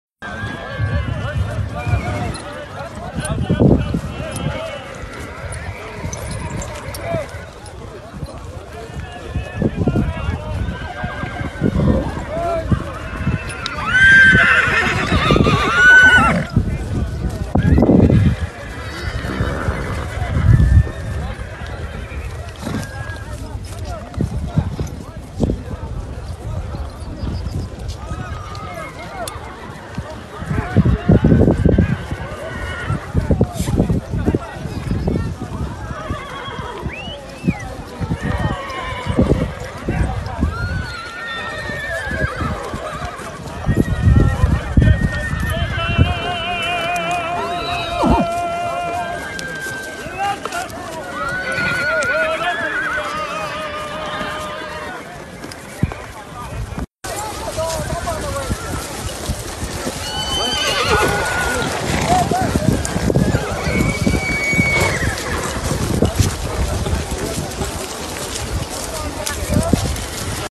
Diriliş Ertuğrul Çeçenya Dağlarında
Diriliş Ertuğrul dizi ekibi, Çeçenya'da düzenlenen ve 1500 atlının katıldığı görüntülerdeki sportif etkinliğe katılarak, Çeçenya dağlarında at sürdüler.